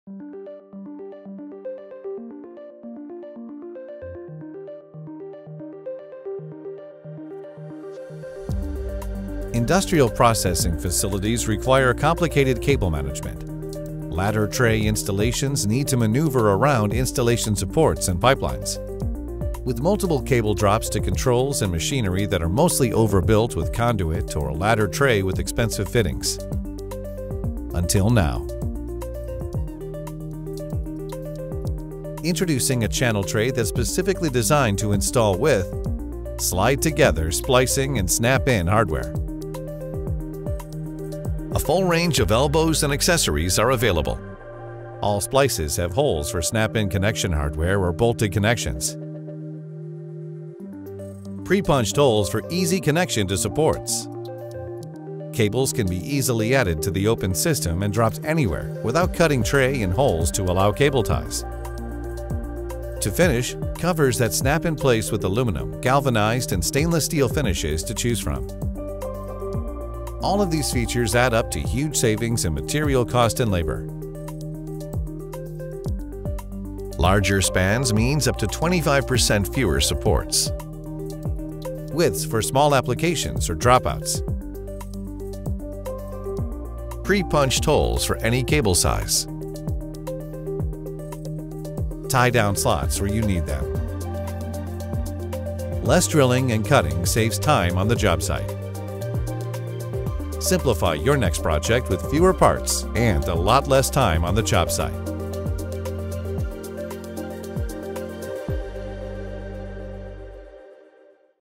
Cable Channel Made Simple
Cut your number of supports by 25% with a complete cable management system featuring quick and easy snap-in hardware.

Ladder tray often needs to maneuver around installation supports and pipelines in industrial petrochemical facilities, and systems can quickly get complicated. Legrand Cable Channel in combination with ITray creates a durable, flexible open system with fewer parts. The unique splice hardware quickly snaps into place, and pre-drilled holes fit cables of all sizes. Less drilling and cutting saves time on the jobsite, and it makes the entire system easier to install, adjust or move.